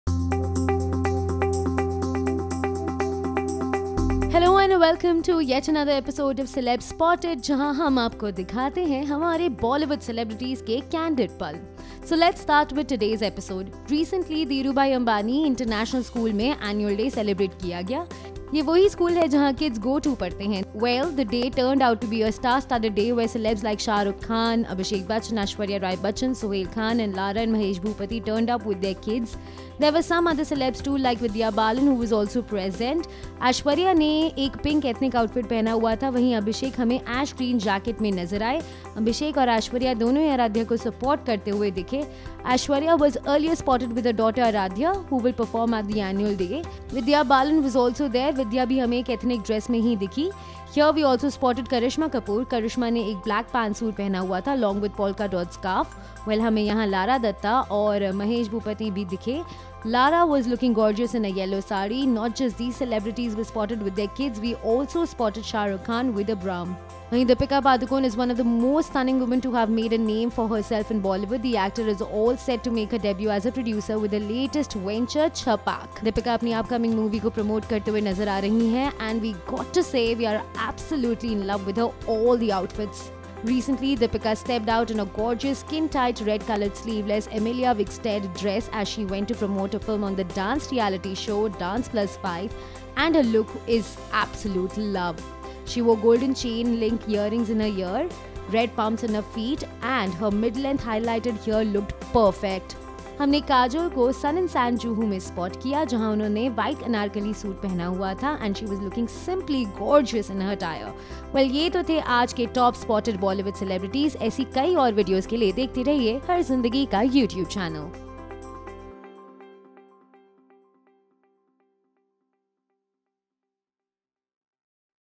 Deepika is all set for Chhapaak’s Promotion
Deepika Padukone is one of the most stunning women to have made a name for herself in Bollywood. The actor is all set to make her debut as a producer with her latest venture, Chhapaak. Ajay Devgn and Kajol is all set to recreate history on the celluloid with his upcoming film Tanhaji: The Unsung Warrior.